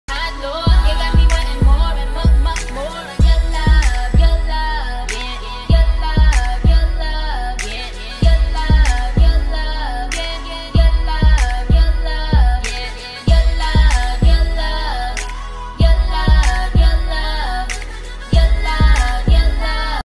Lace front straight hair wig install~ Without drama baby hair style, do you like it?nadula shorts
❤Use the Special Code get EXTRA $10 OFF: nadulatb